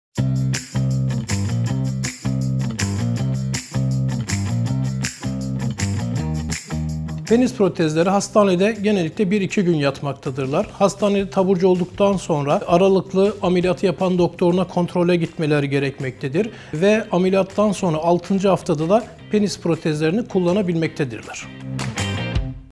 Penis Protezi Ameliyatı Sonrasında Hastanede Kalış Süresi Ne Kadardır? – Prof. Dr. Tolga Akman
Medicana Bahçelievler Hastanesi Üroloji bölümünden Prof. Dr. Tolga Akman “Penis Protezi Ameliyatı Sonrasında Hastanede Kalış Süresi Ne Kadardır?” sorusunu sizler için yanıtladı.